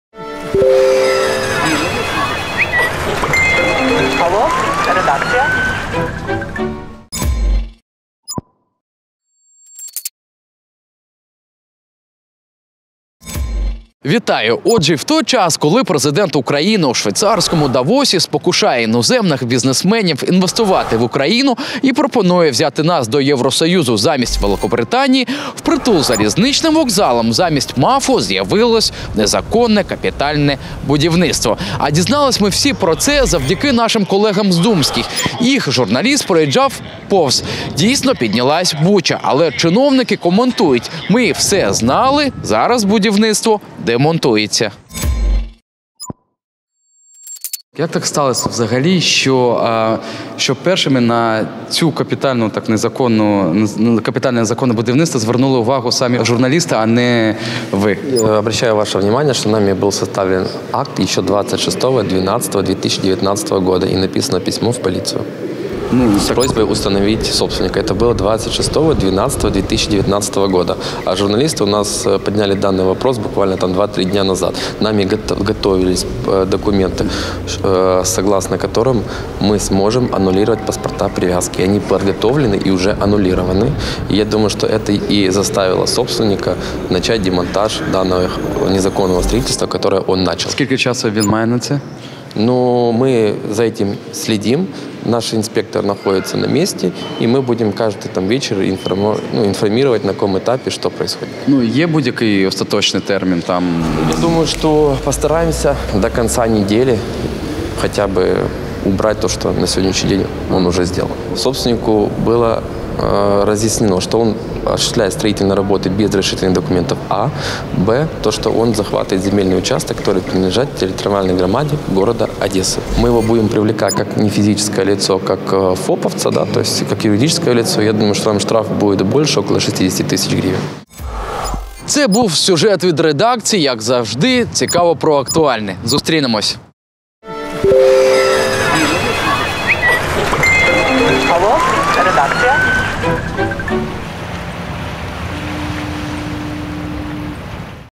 Капитальную стройку вместо будок возле ЖД Вокзала заставят снести самого хозяина заведения
Медіапроєкт
TV:              Телеканал А1